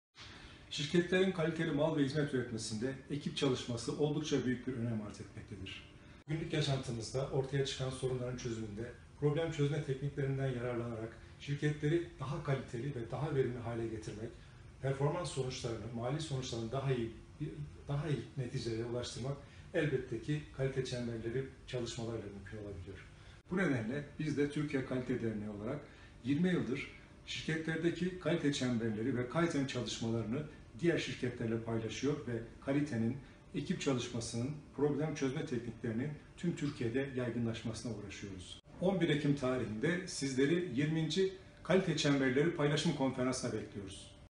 KalDer Ankara Şubesi Yönetim Kurulu Başkanı Nazmi Karyağdı, 20.Kalite Çemberleri Paylaşım Konferansı
20. Kalite Çemberleri Paylaşım Konferansı 11 Ekim'de 1.OSB Bölge Müdürlüğü Konferans Salonu, Ankara'da